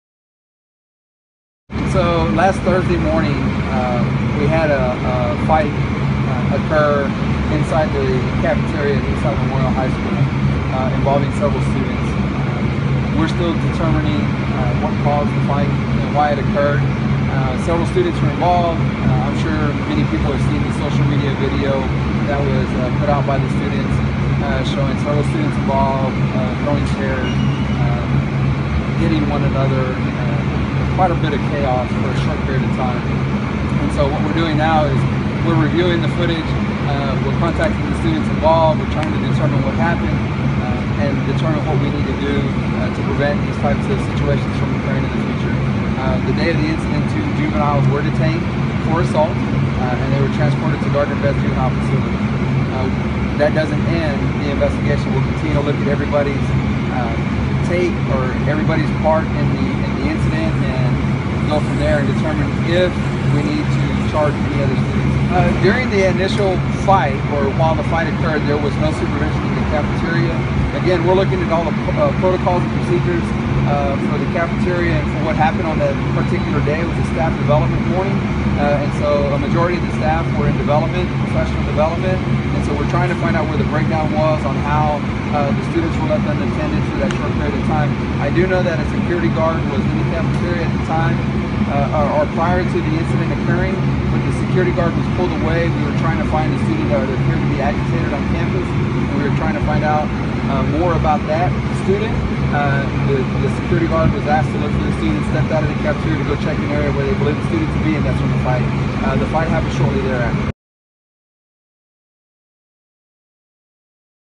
AISD responds to fight at Eastside Memorial High School
Police said that a brawl broke out in the Eastside Memorial High Schoo cafeteria shortly before 10 a.m. on April 9, 2015.